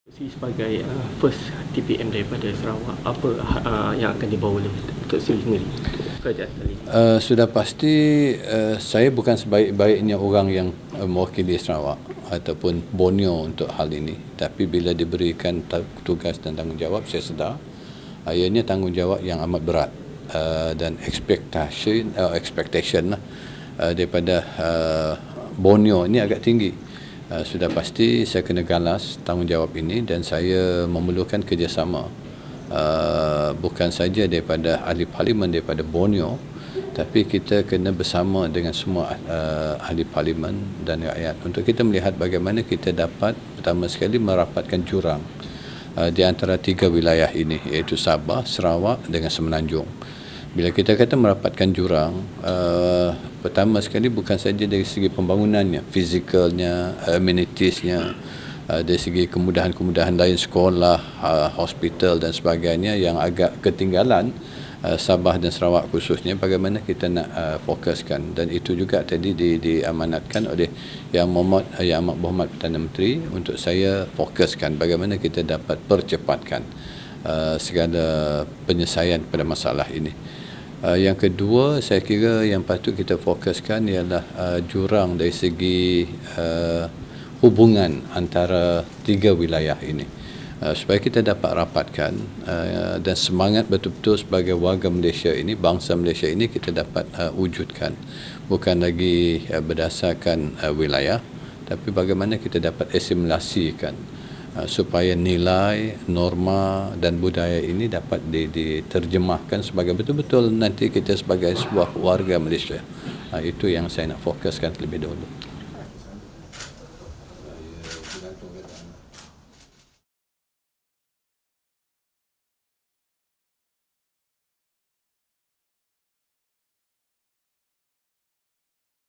DPM Fadillah to focus on fostering closer ties between Sarawak, Sabah and Peninsula
Deputy Prime Minister Datuk Seri Fadillah Yusof on Monday (Dec 5) said his focus will be on implementing various initiatives to foster closer relations between Sarawak, Sabah and the peninsula.

Fadillah, who is also Plantation and Commodities Minister, said the initiative to bridge the facilities and development gap in Sarawak and Sabah was also his main concern.